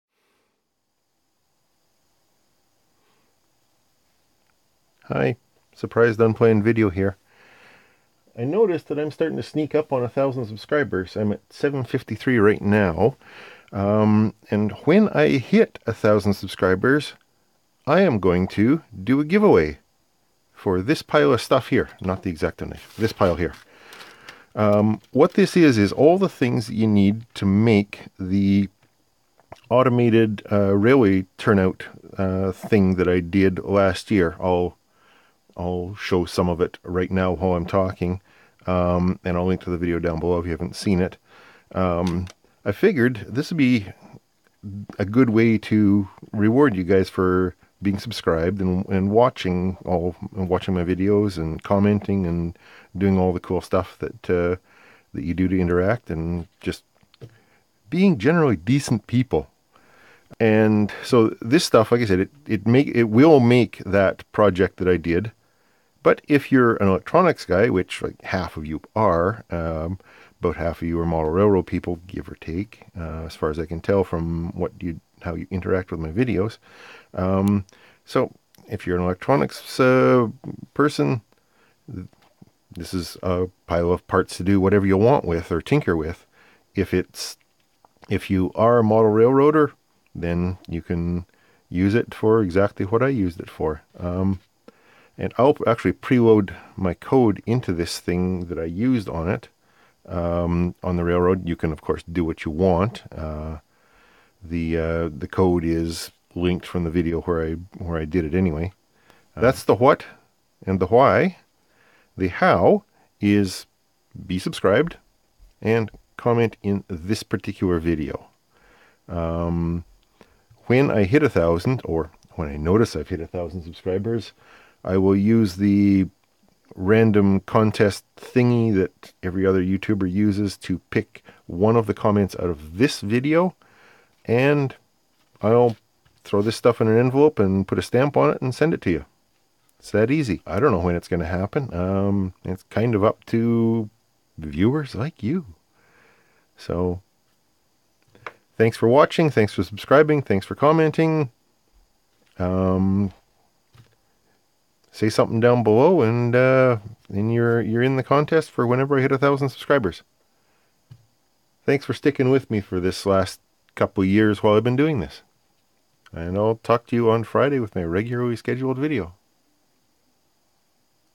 Giveaway Announcement
If you want to win, here's what you do:
2: comment on this video.

When my channel hits 1000 subs, I'll use an automatic chooser thingie to pick one comment form this thread and give them the stuff in this video. (If the current trend holds, that should be sometime in early March)

As with all of my videos, this isn't the only way to do it.
It's probably not the best way to do it.

But it's how I do it.